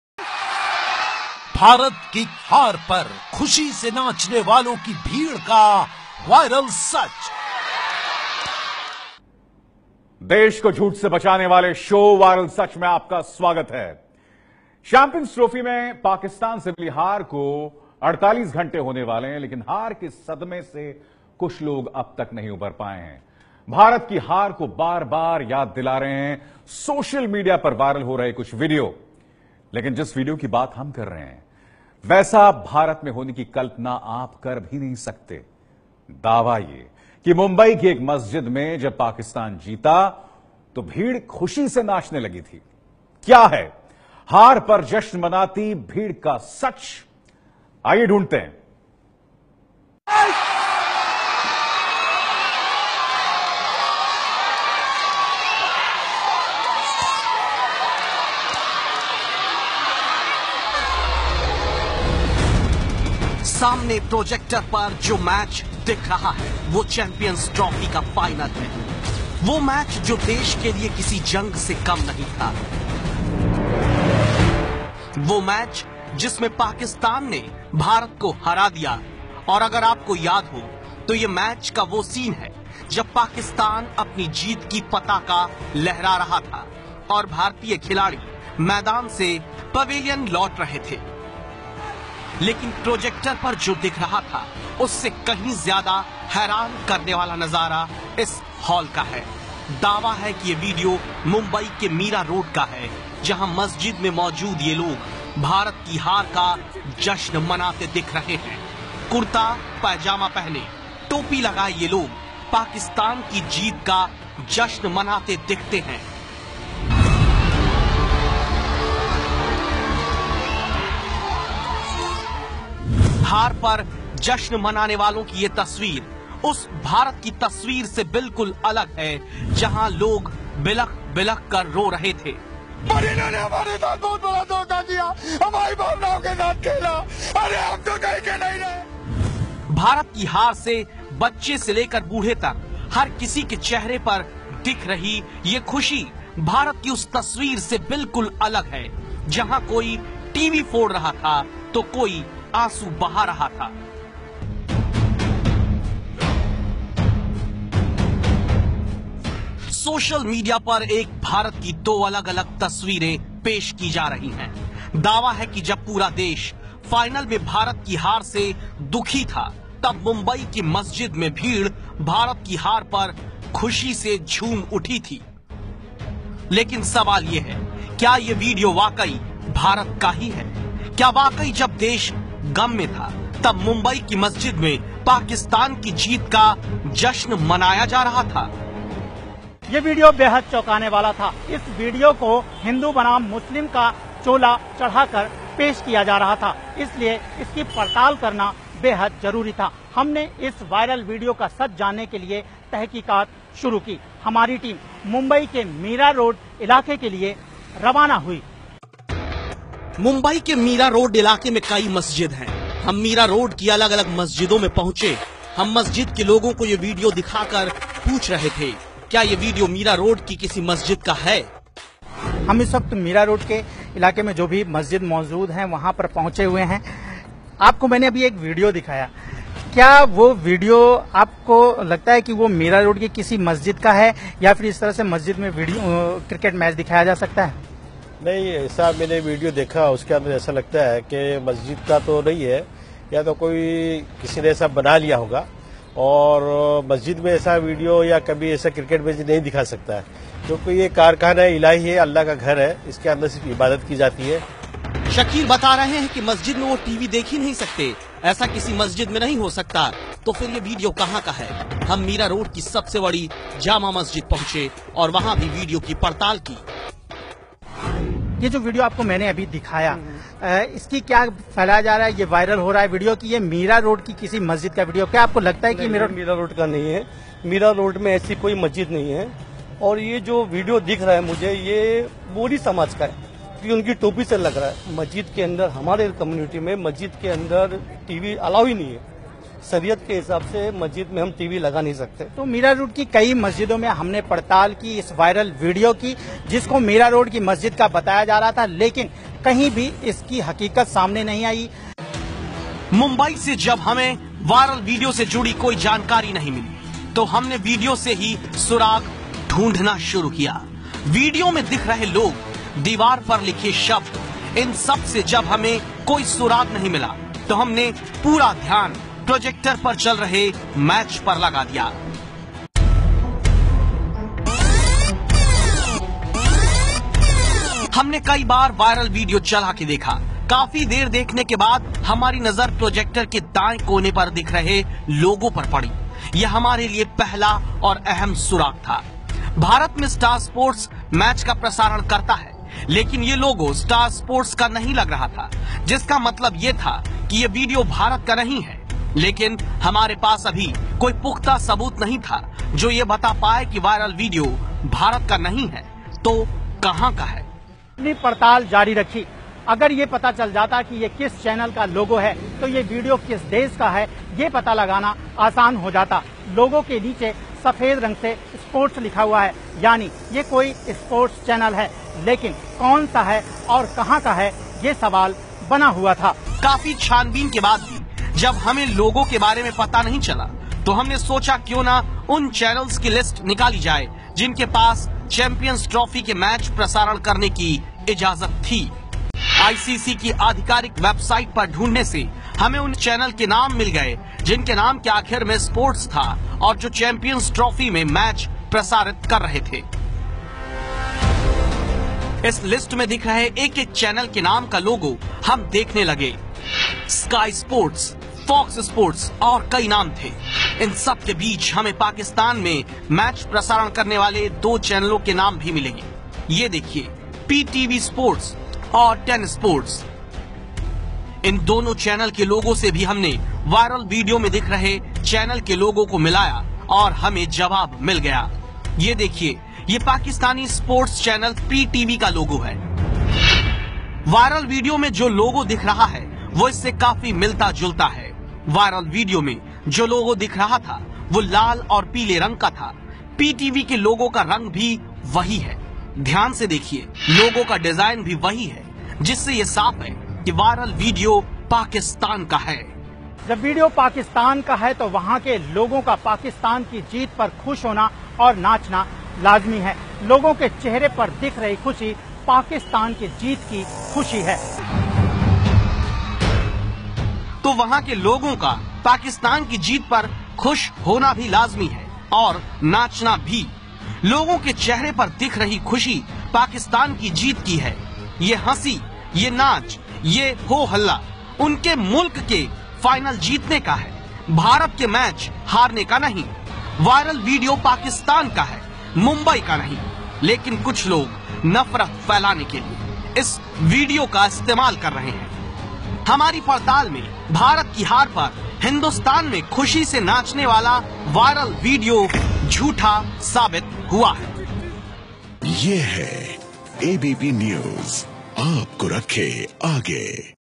Mira road mosque fake video
Please first confirm than hate muslim this the truth of mira road mumbai cricket celebration Pakistan win over india